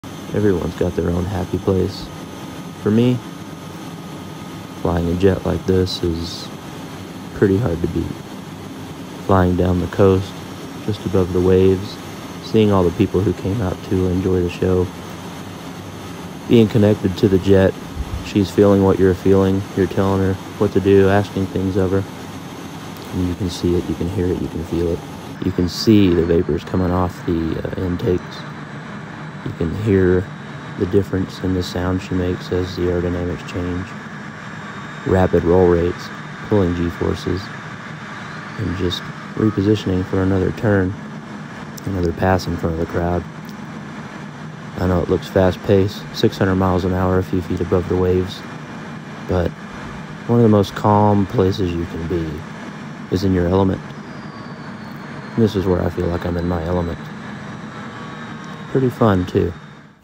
Huntington Pier JET FLYOVER at 600mph
@jerodflohr piloting a TA-4 Skyhawk during the 2022 @pacificairshowofficial